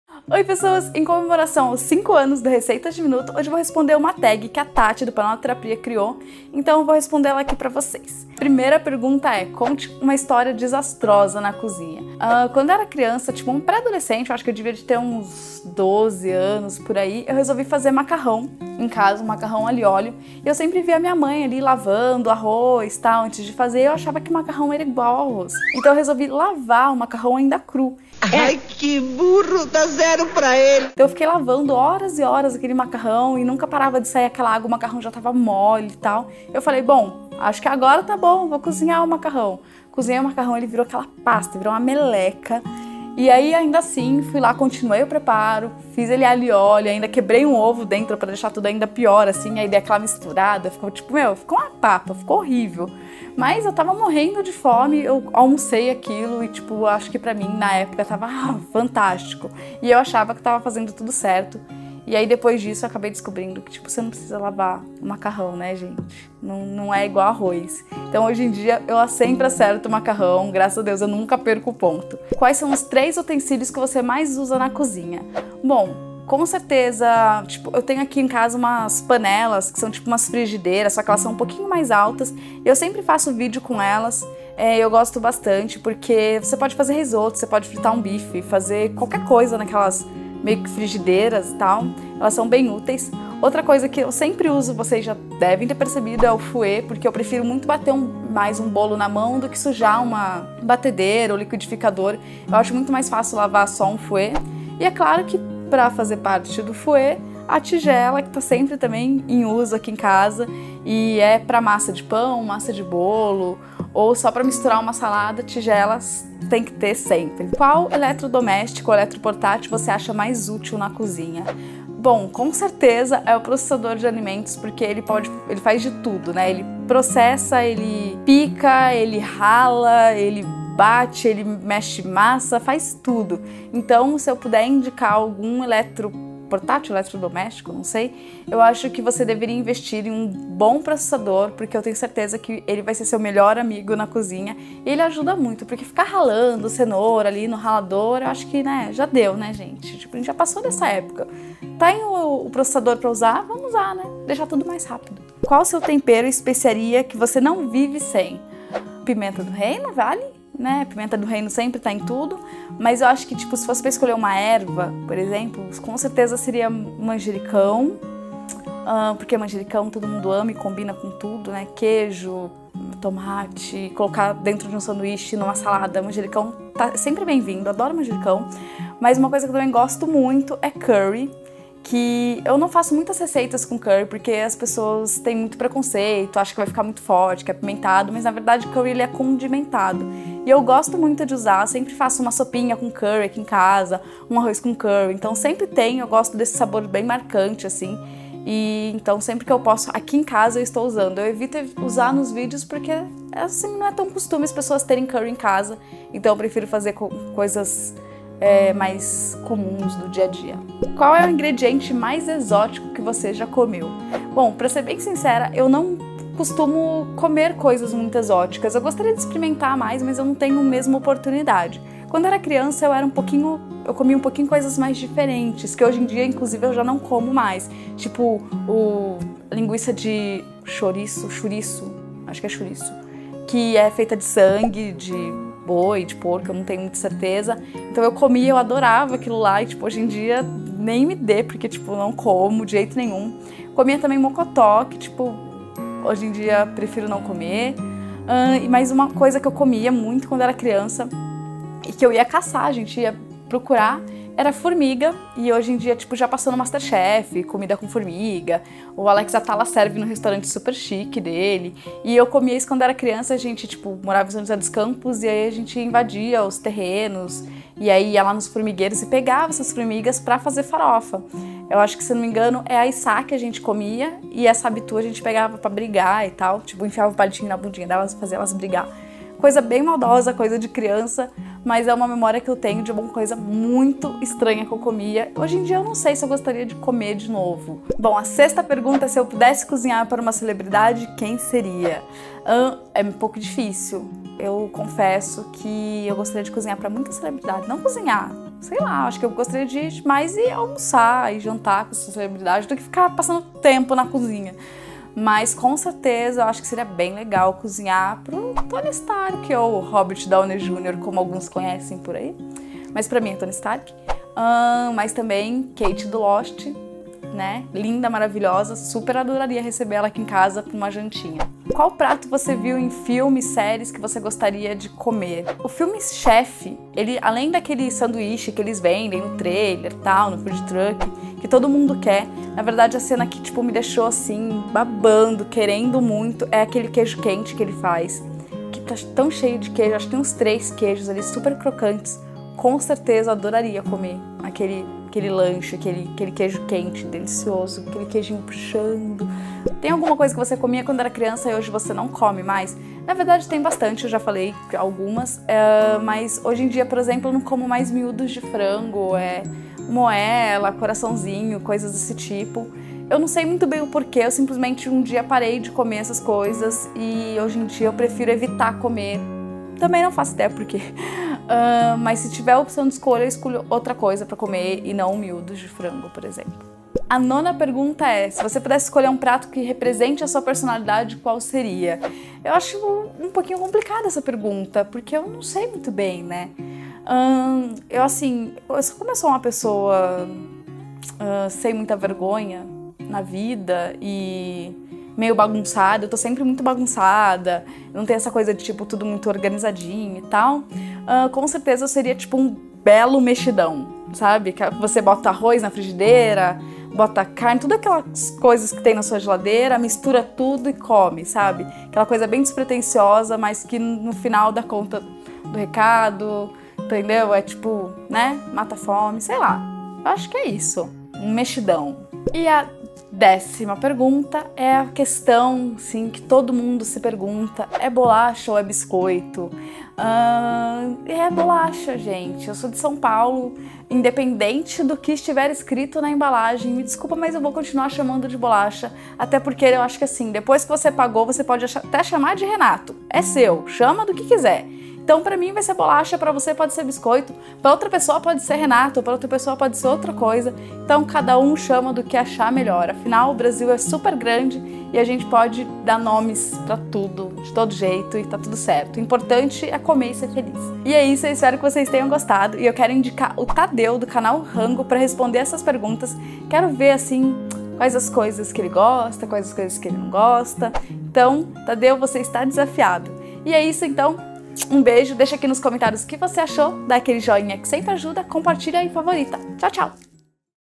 TAG: Curiosidades de Cozinha
Nesse vídeo respondo algumas perguntas sobre utensílios que costumo usar, temperos e coisas estranhas que já comi.